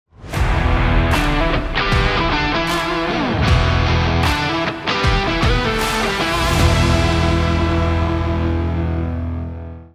AIn't Got The Work Ethic | Work boots for the irreplaceable worker.
Duradero work boots come with a free resole, so they last twice as long as other work boots. Human ingenuity at its finest.